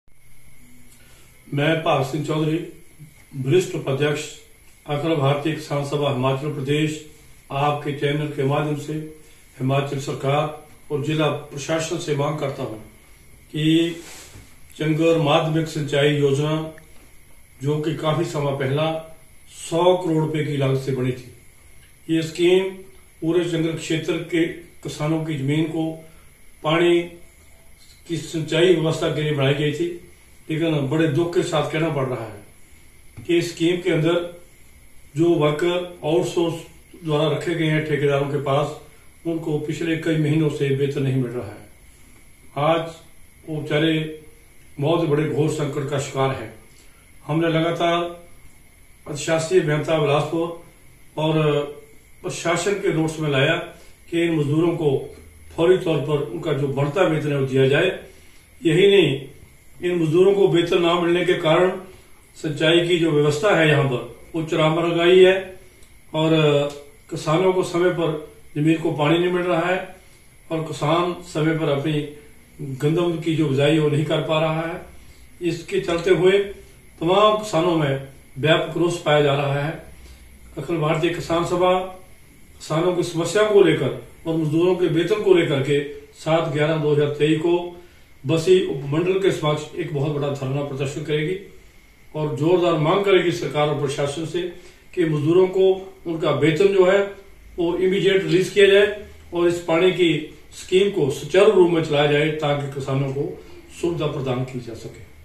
Himachal | मजदुरों के वेतन और किसानों की समस्याओं को लेकर कल बस्सी में होगा विशाल धरना प्रदर्शन.....
There will be a huge protest in Bassi tomorrow regarding the wages of workers and the problems of farmers.